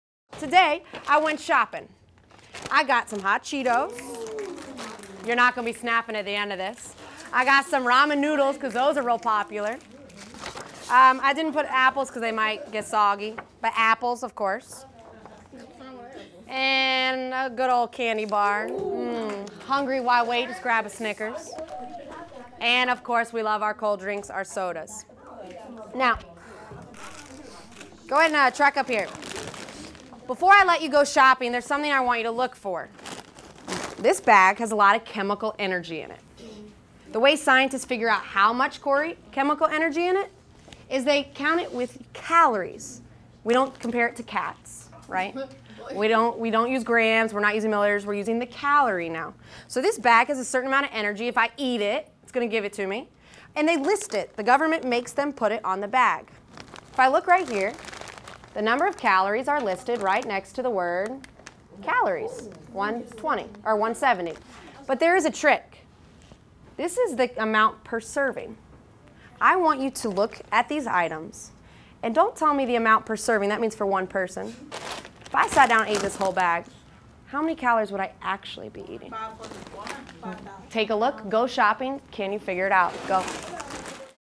6th Graders Count Calories at KIPP Central City Academy
At the time this video was taken, Hilah Barbot was a 6th grade science teacher at KIPP Central City Academy in New Orleans. In this video, Hilah hooks students into a lesson on evaluating a person's health by the number of calories they consume.

See the KIPP Framework for Excellent Teaching to learn more about what we believe makes an excellent teacher. (This video relates to Classroom Culture)